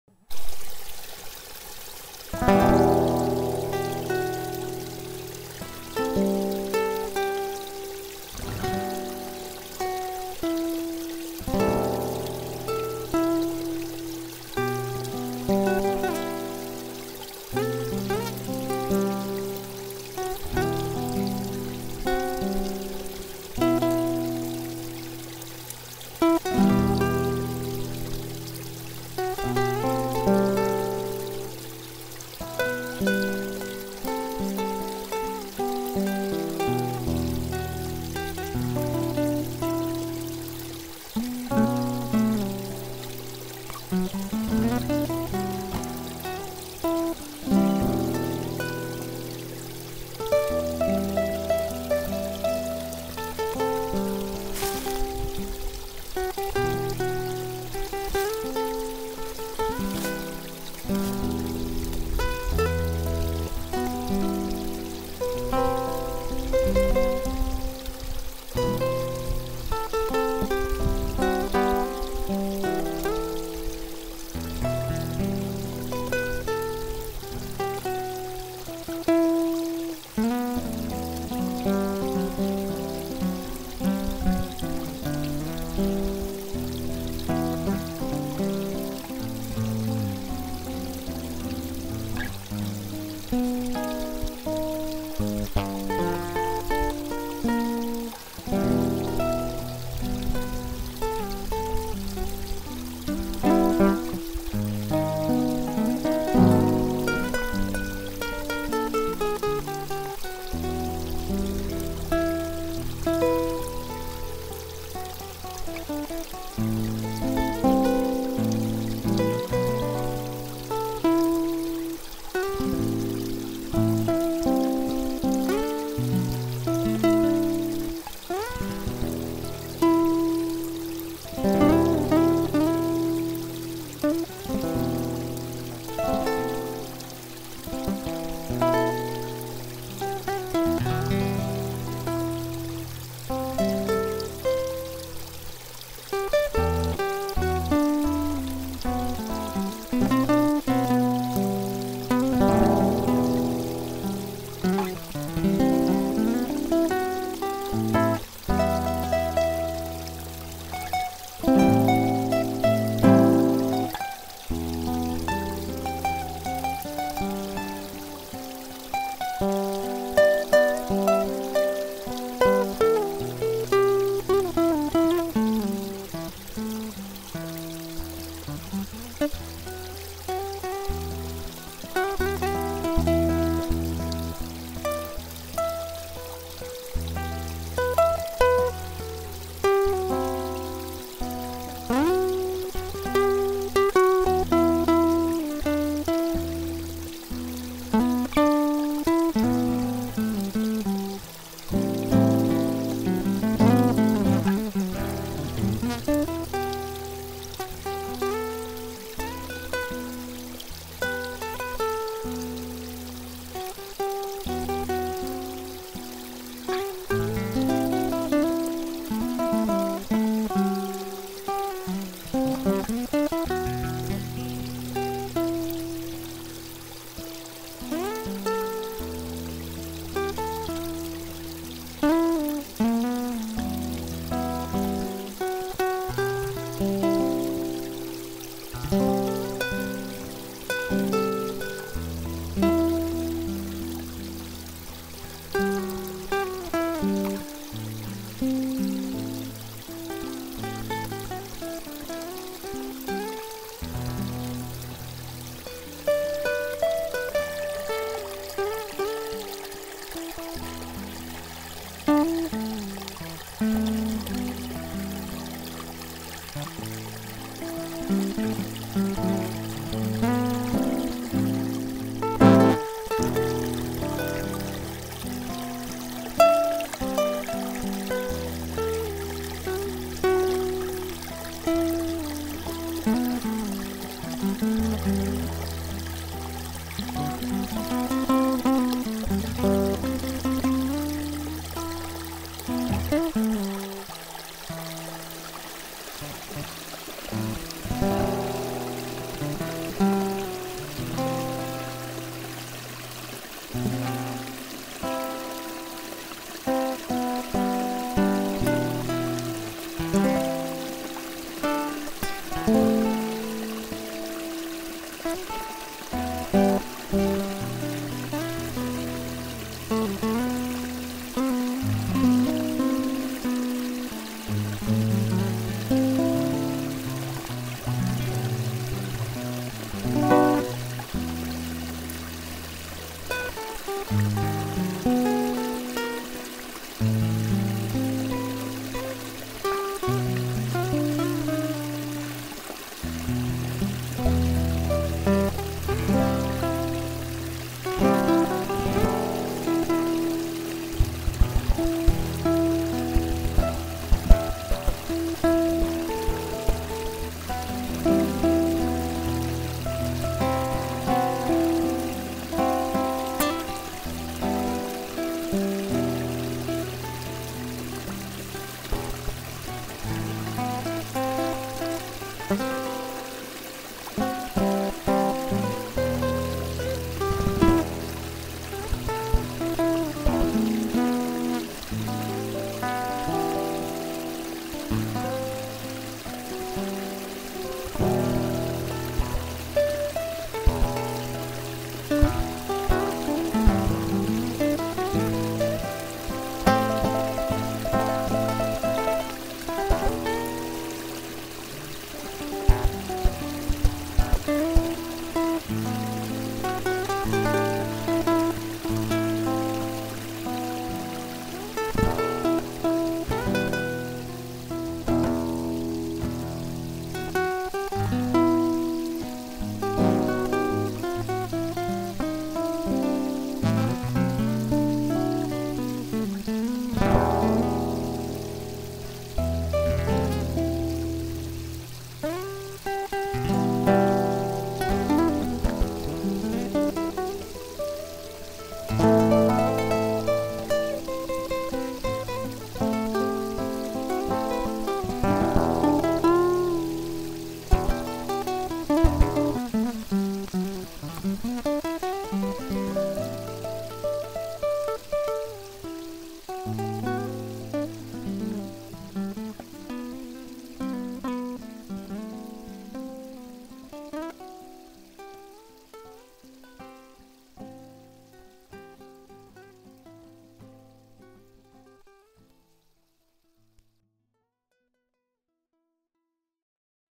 Muzyka relaksacyjna January freestyle tomp
Gitara prosto z mojego wnętrza, zdjęcia wykonałem w mojej okolicy, miłego słuchania.